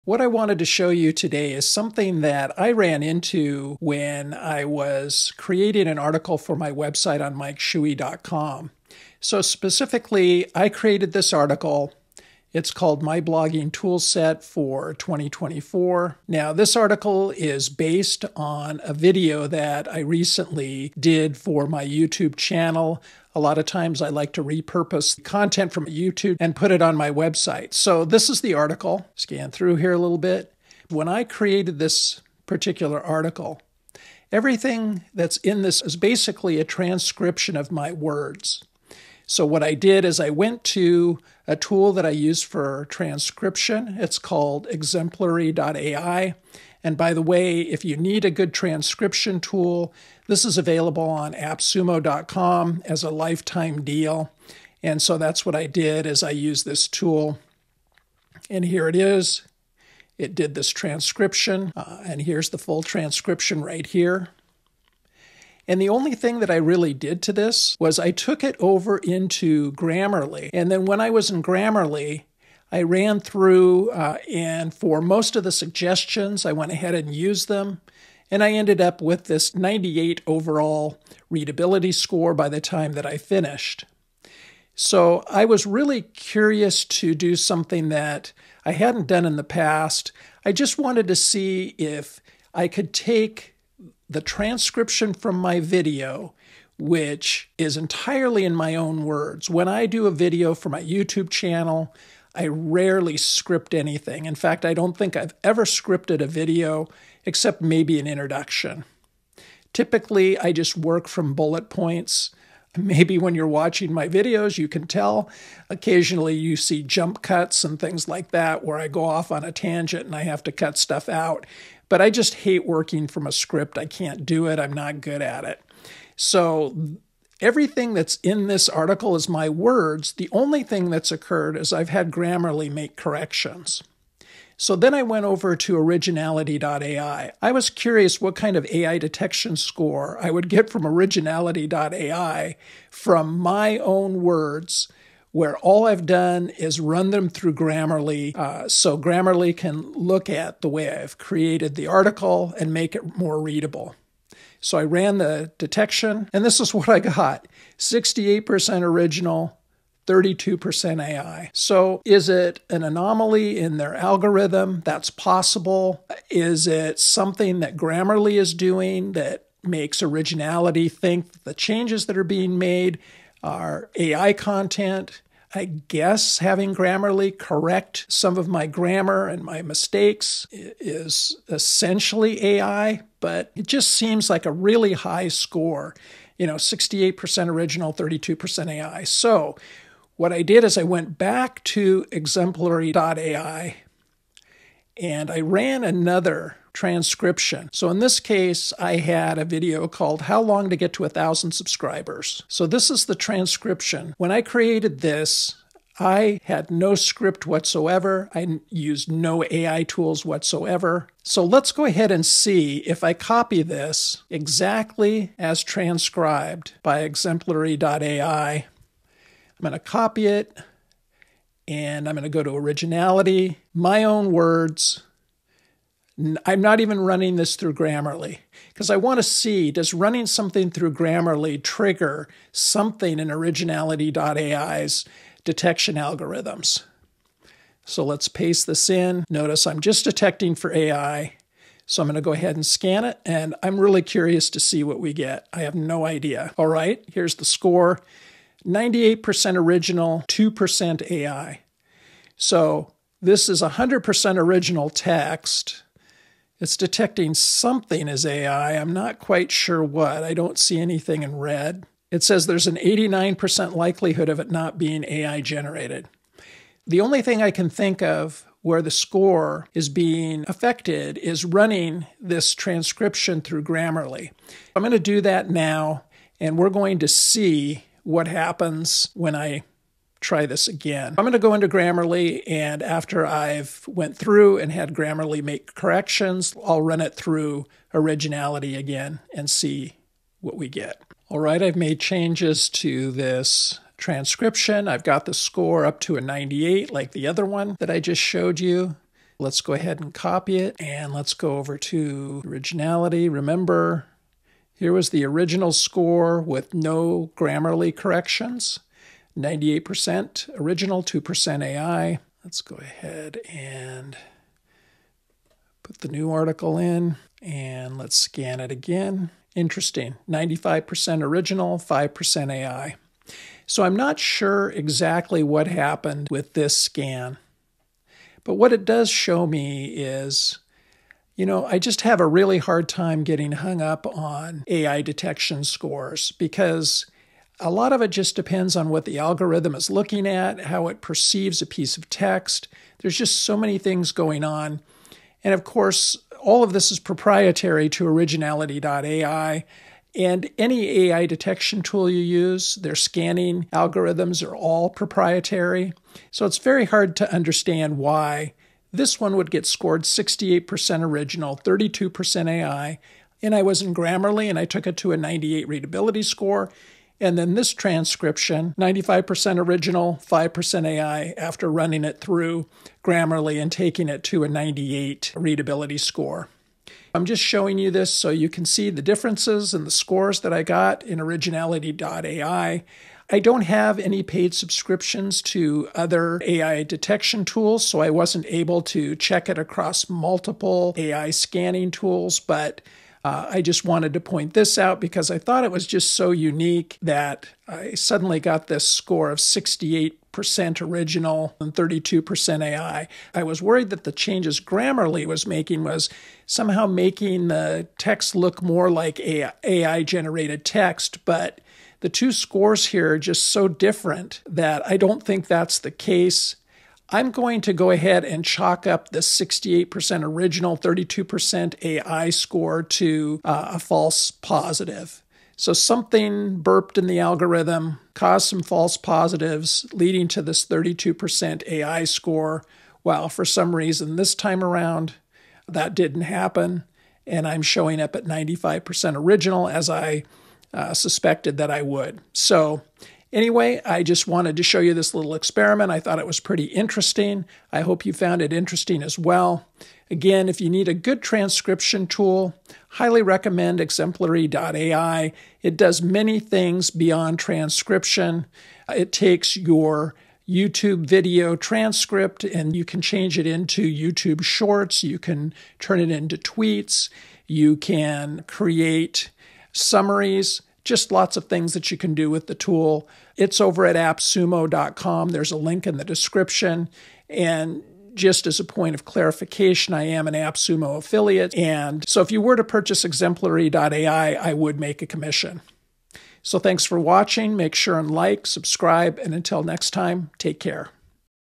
How accurate is AI detection? You might be surprised!
How much faith should you have in AI detectors? While I believe they are pretty accurate, they can get it wrong, too. Enjoy this AI detection experiment!

Need a good transcription tool? Check out:
Exemplary.ai @ AppSumo.com:

SEO OPTIMIZATION:
NeuronWriter for SEO optimization:

Find great keywords fast:
KeyWords People Use: